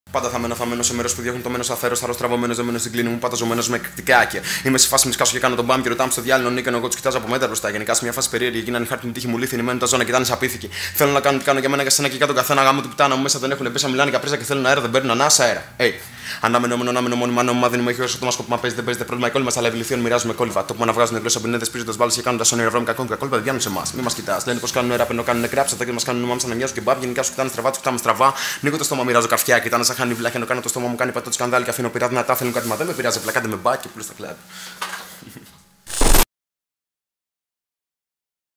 Ακροάσεις Schoolwave 2019 - TEASER 2
Εσύ πόσες λέξεις μπορείς να πεις με 5 ανάσες;
►Σύντομα έρχεται και ολόκληρο το backstage video από τις ακροάσεις της Αθήνας και της Θεσσαλονίκης.